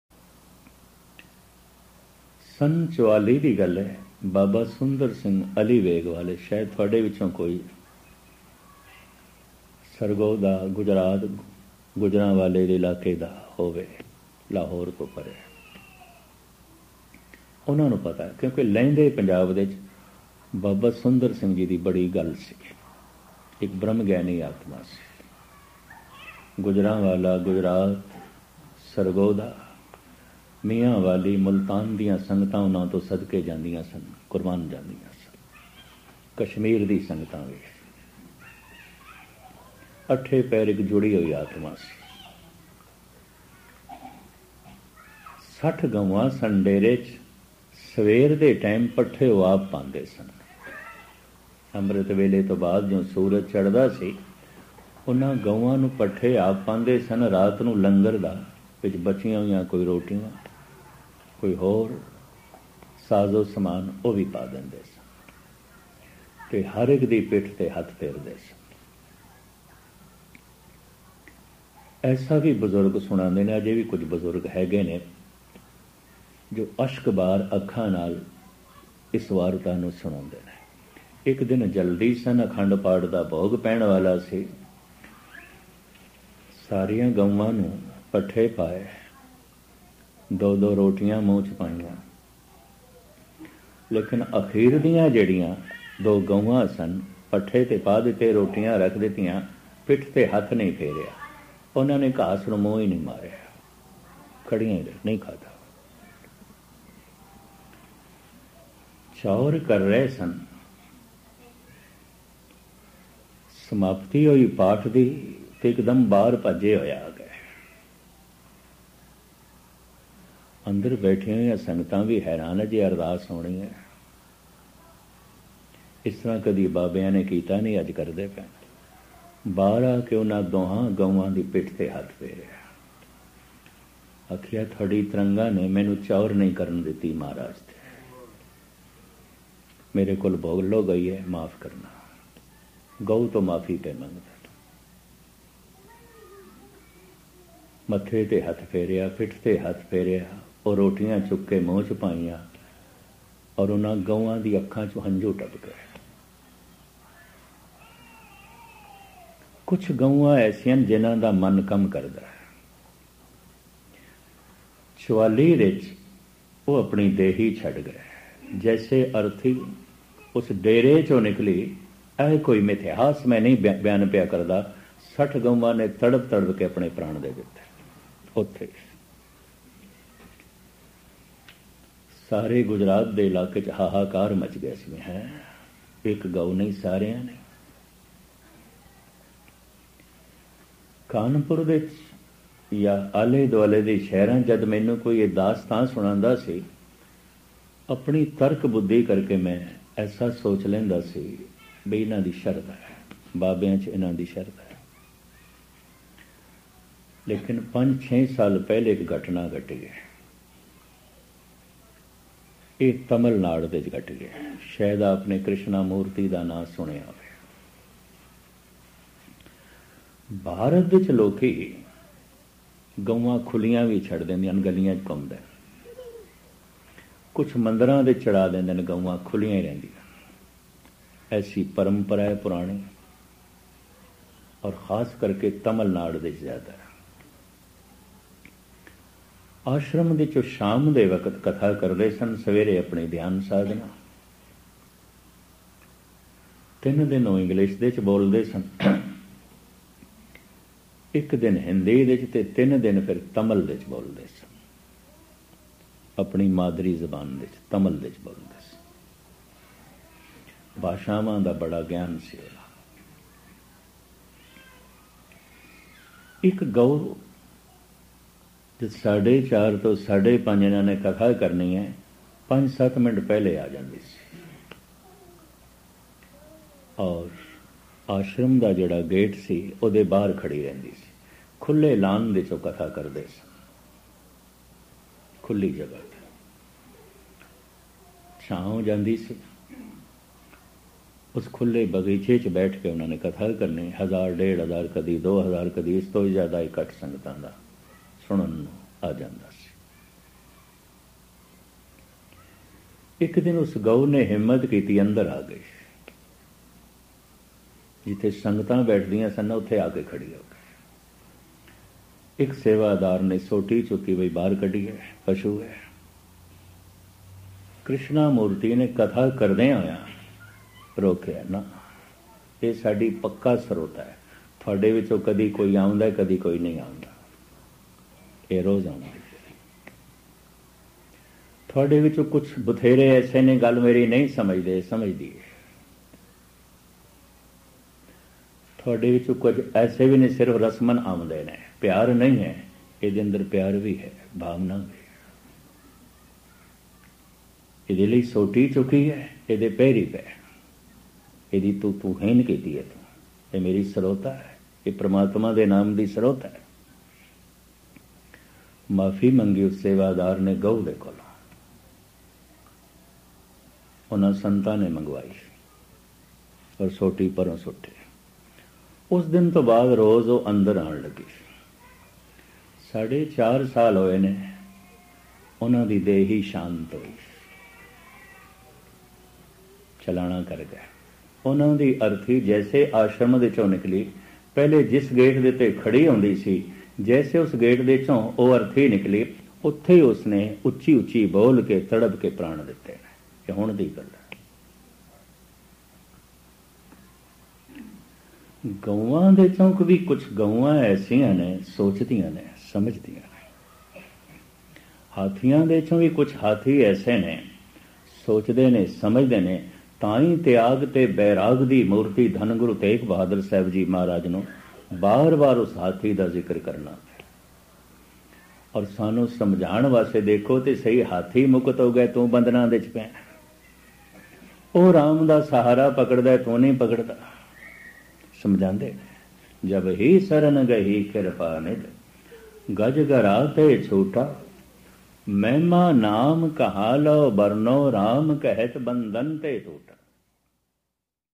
Ek Gau Jo Roj Katha Sunan Lai Aundi C. Te kyo Is Gau Ne Tadap Tadap Apni Jaan De Ditti.
About
sant singh ji maskeen this katha

Dass Vicky. Presents..

Tital. Ek Gau Jo Roj Katha Sunan Lai Aundi C. Te kyo Is Gau Ne Tadap Tadap Apni Jaan De Ditti.

Katha By.  ਸੰਤ ਸਿੰਘ ਜੀ ਮਸਕੀਨ..

Y
Lebal. Dass Vicky